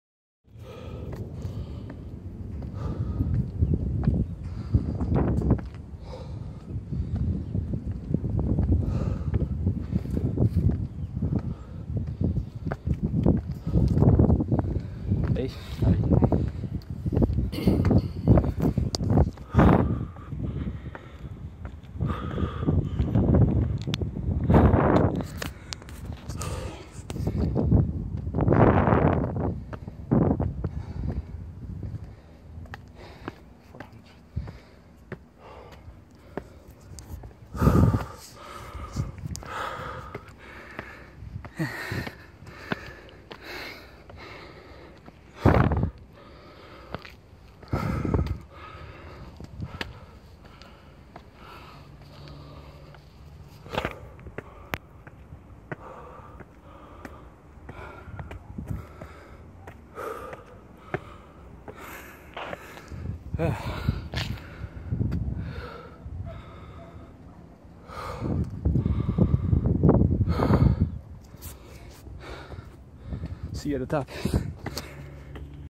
Climbing Hooiberg
Short video depicting the steep climb to the top of Hooiberg  in Aruba. My total time that day was about 7 minutes, my personal best ever was 4 minutes 53 seconds.